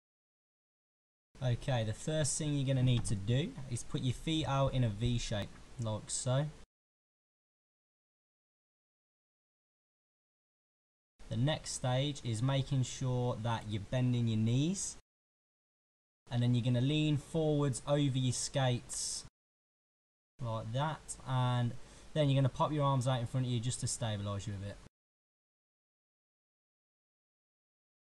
How to stand up - ice skating
How to stand up on the ice with the correct posture while ice skating

MrTheWebby - voice over